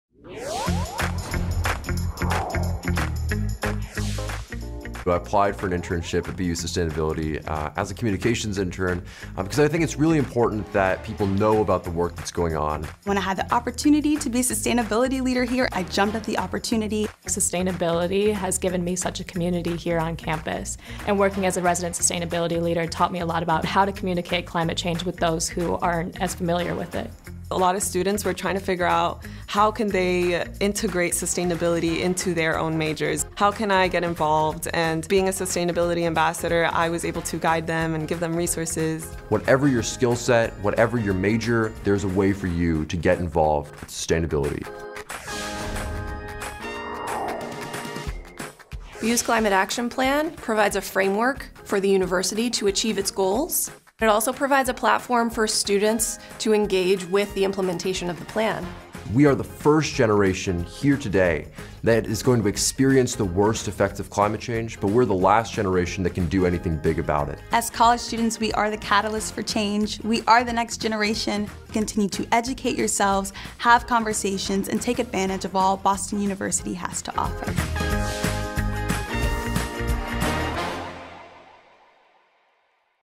Sustainable Living in College | How to Get Involved at Boston University
Boston University approaches sustainability through the interconnection and interdependence of environmental, social, and economic factors, and our students have ample opportunity, both on and off-campus to get involved. Educate yourself about BU’s Climate Action Plan and the progress toward the goals. Integrate sustainable actions in your daily life. Talk with your friends, classmates, and professors about sustainability. Take advantage of the many programs and initiatives available to you. Where you see opportunities to make improvements, share those and be a leader.